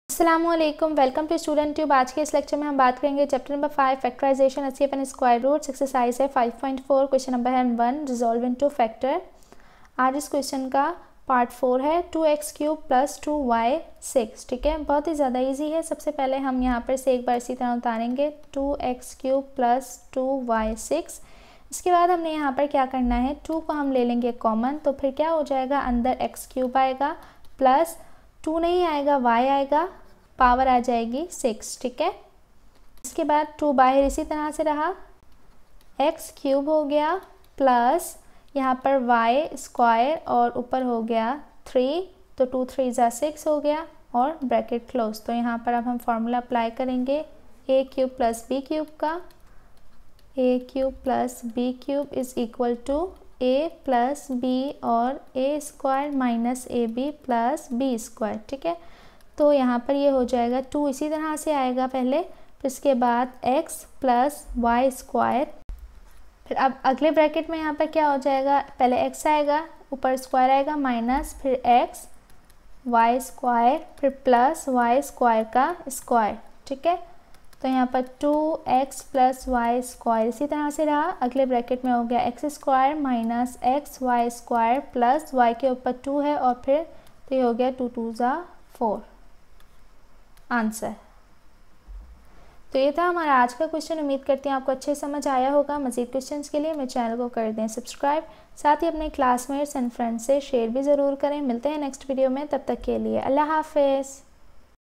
Chapter 5 - Exercise 5.4 - Q 1 part 4 Solution (Class 9 & 10 Maths - Sindh Board Education)
Today's student tube lecture is about the chapter 5 - Exercise 5.4 - Q 1 part 4 Solution (Class 9 & 10 Maths - Sindh Board Education) Resolve into factor?

and

detail.
explain another topic feel free to write us in the comment section.

On Student Tube, we will cover Maths, Accounts and English for 9th, 10th, 11th, 12th and University students.